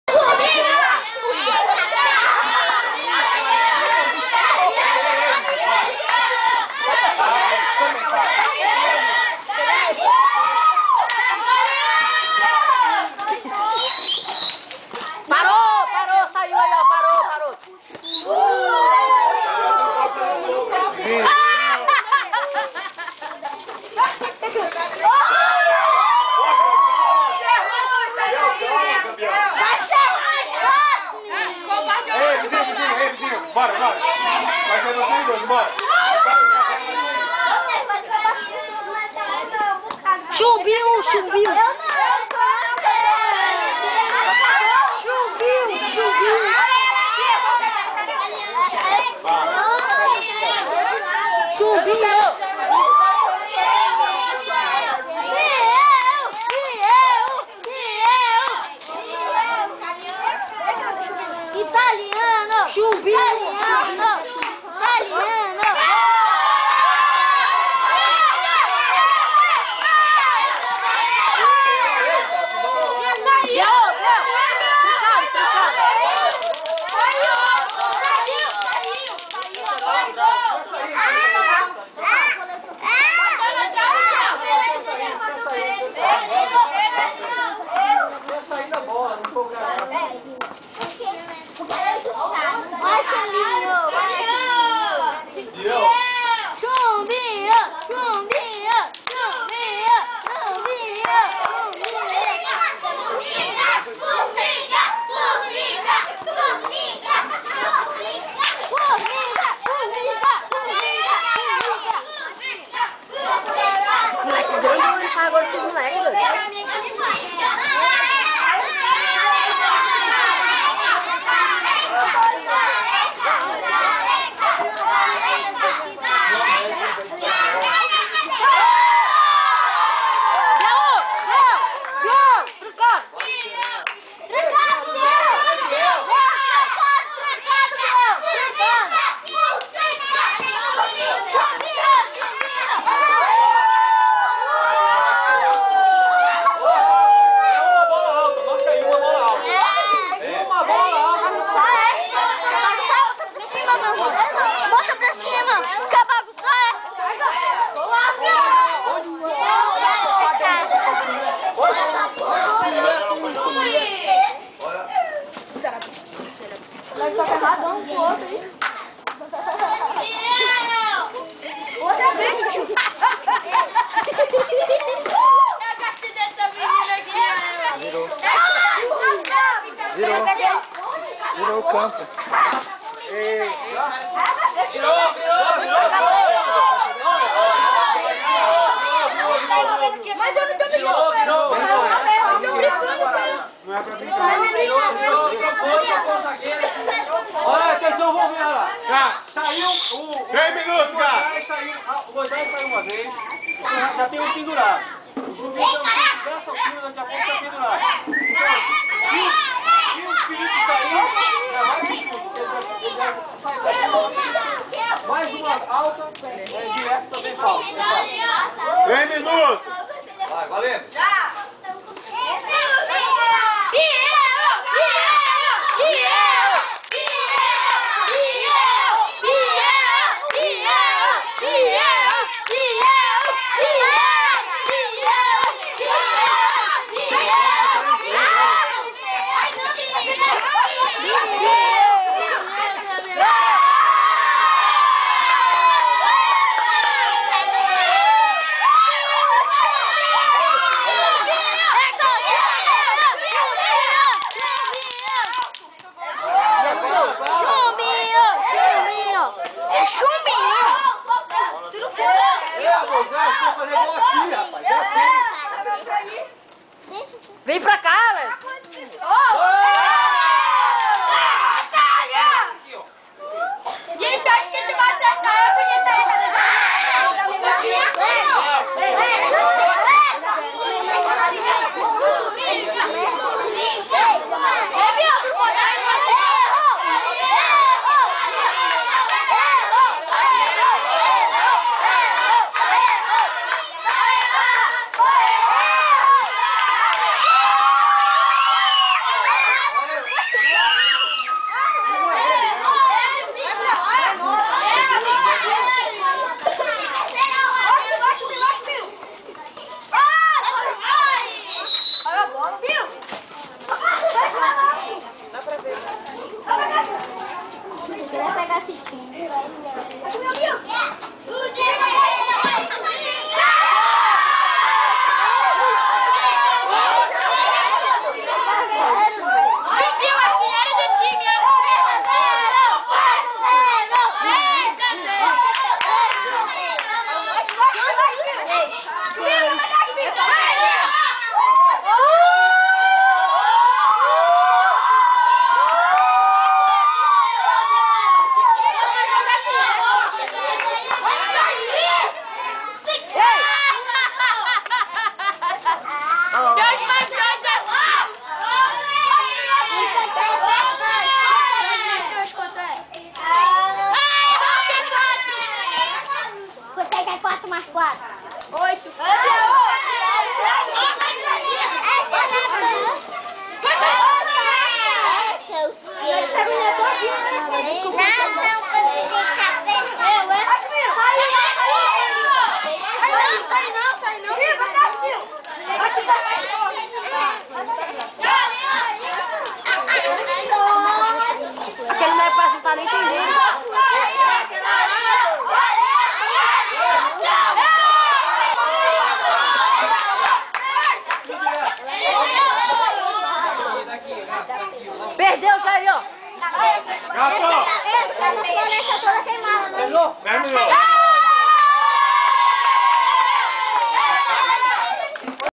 Torneio de Pebolzin da Afin- 1a rodada.MPG
Mais uma partida do torneio de Pebolzin da Afin, onde as crianças brincam na produção afinada feita pelas crianças afinadas com régras próprias diferente da pelada tradicional. Uma delas é que só os zagueiros podem circular o campo todo e fazer gols, enquando os atacantes ficam limitadas a duas linhas que divide o meio campo e só armam a jogada.

Partida: Biel e Kalian Chumbinho X Felipe e Moises